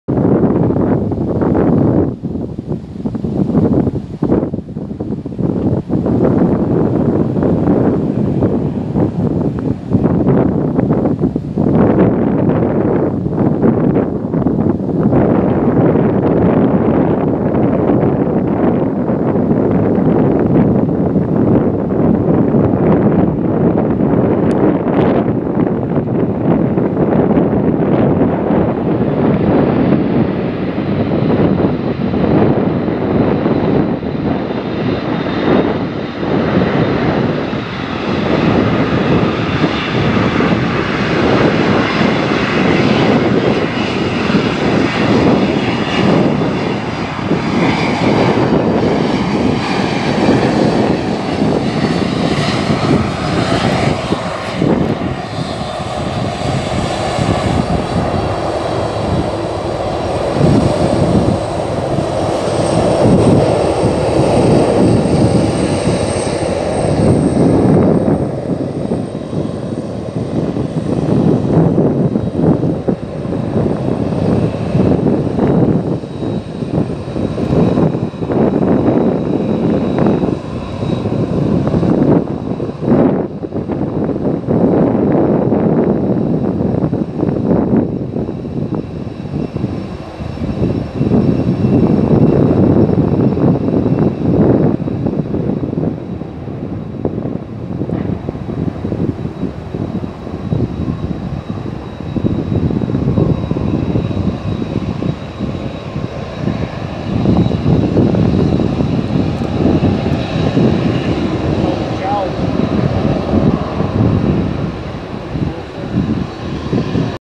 Gorgeous Kalitta Air Boeing 747 landing at East Midlands from Brussels!!
A lovely Monday visitor here in the form of N743CK Kalitta Air 747-400F, seen arriving at East Midlands Airport from Brussels as CKS244, which later departed to Cincinnati!